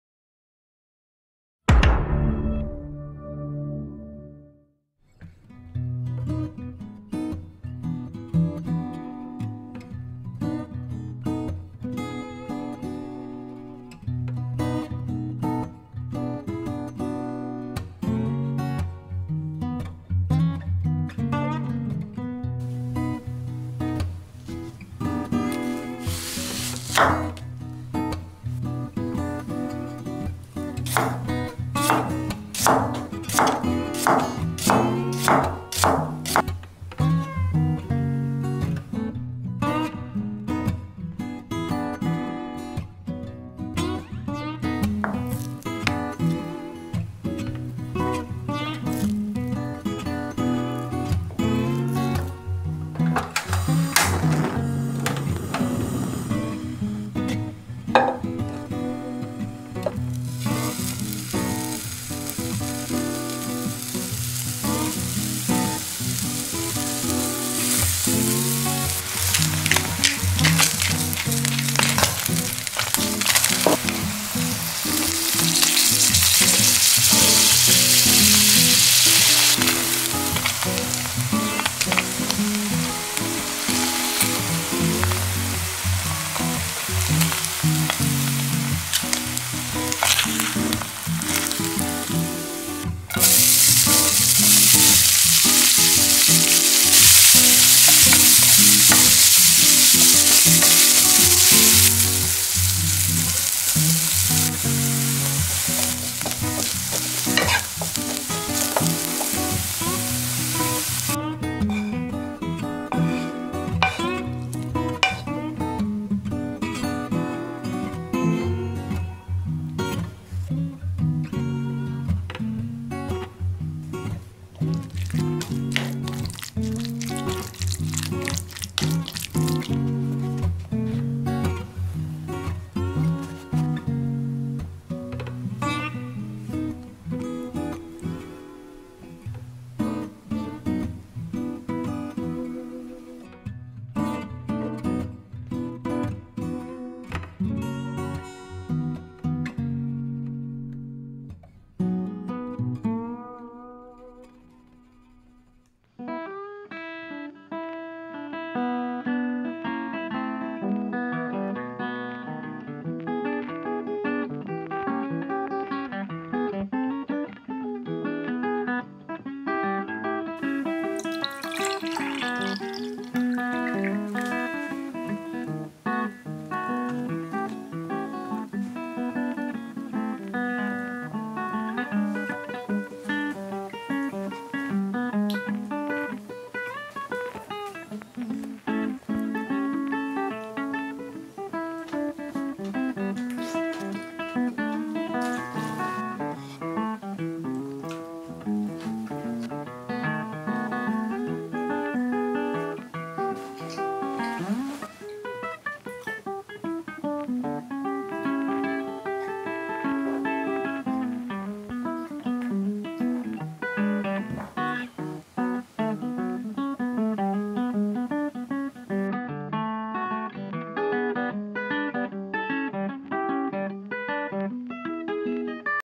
스팸 양파 샌드위치/ Spam Onion Sandwich/how to cook /recipe#52
스팸 양파 샌드위치

재료

모닝빵 4개

양파 2/1개

스팸4조각

마요네즈1테이블 스푼

딸기잼1테이블 스푼

버터10g

후추 약간

Spam Onion Sandwich

Ingredients

4 morning bread

2/1 onion

4 pieces of spam

1 tablespoon mayonnaise

1 tablespoon strawberry jam

10 g butter

Some pepper